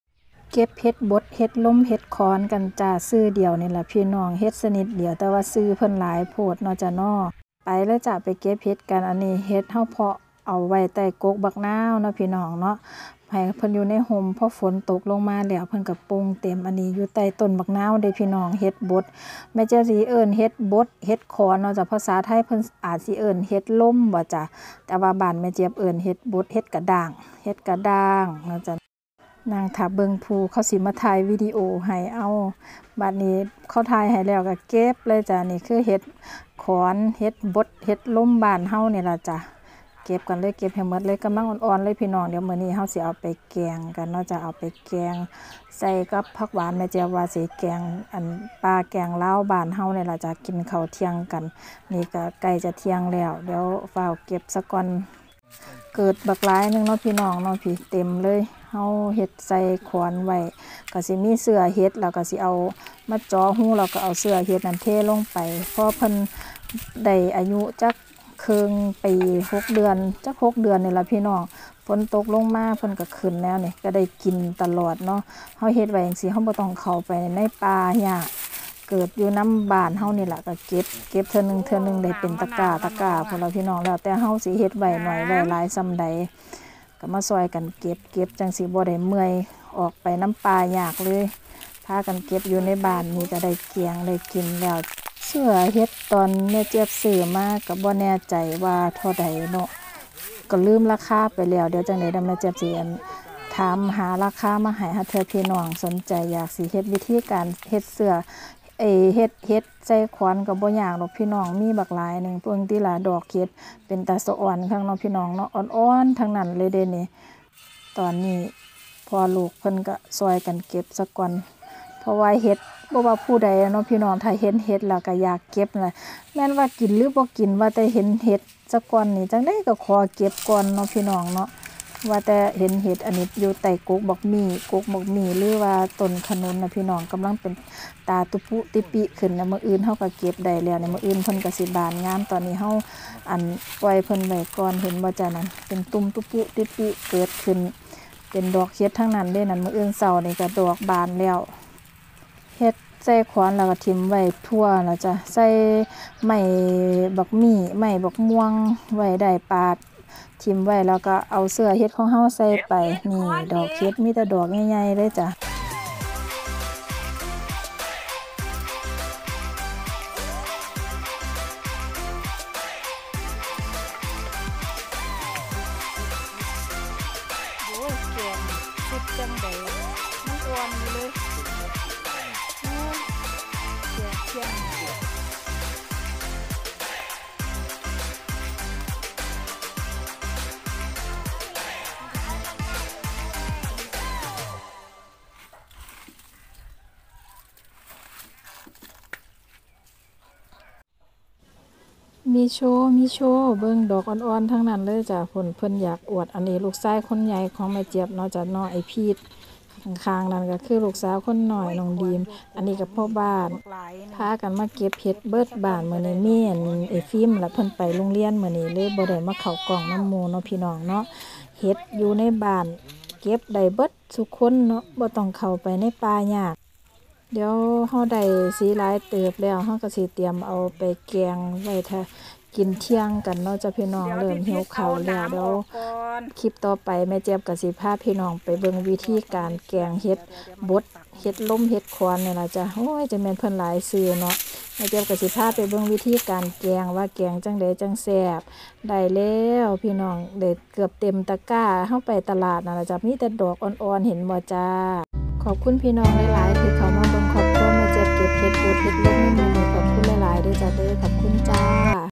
วิธีเก็บเห็ดลมอยู่ติดหัวบันไดบ้านByชนบท บ้านฉัน
พาเก็บเห็ดลมหรือว่าเห็ดบดอยู่ติดกับบันไดบ้าน
ชีวิตดี๊ดีเราไปดูวิธีการเก็บเห็ดเลยค่ะซึ่งเห็ดลมเห็ดบดนี่
เราได้ปลูกไว้เฉพาะไว้ที่ติดกันกับโรงครัวที่ห้องครัวครัวบ้านแม่เจี๊ยบเลยนะคะ
เราไปดูวิธีการเก็บเห็ดลมเยอะๆเลยค่ะวันนี้มีแต่ดอกอ่อนๆทั้งนั้นเลยจ้า